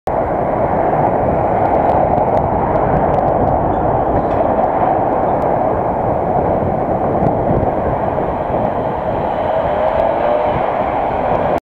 Close pass Purdy contracts
Very close pass from a Purdy Contracts boiler engineer
Registration LB15 FDP. date 10.08.17 Time 08:50